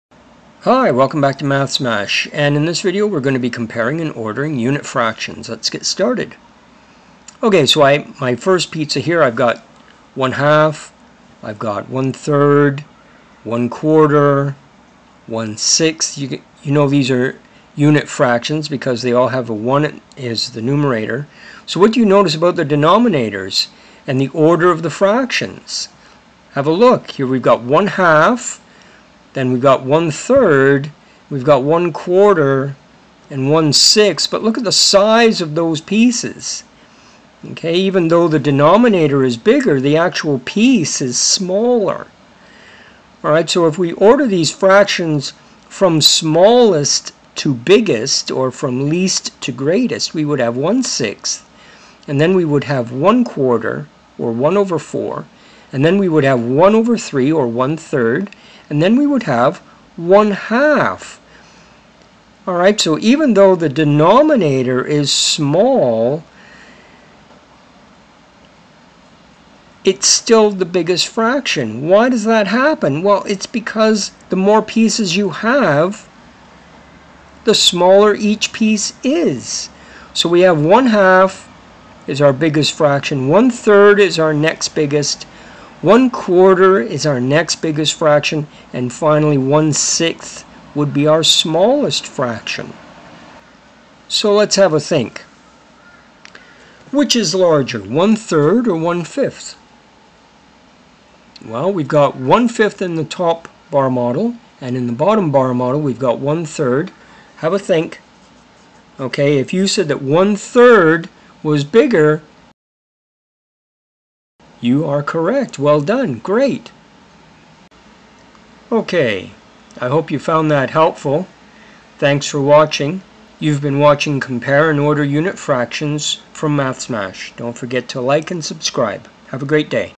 Compare and Order Unit Fractions
In this video, through the use of bar models and pizzas (yum!) we look at comparing some basic unit fractions and then order them according to size. We learn the rule that the larger the denominator the smaller the fraction.
I answer these questions in the video:
What do simple unit fractions look like in pictures?
How can we compare unit fractions?
How can we order unit fractions?

Chapters

00:00 Introduction
00:10 Unit fraction pizzas
00:25 What do you notice about the denominators and the order of the fractions?
01:12 Ordering unit fractions from greatest to least
01:40 Concept checking question
02:05 Closing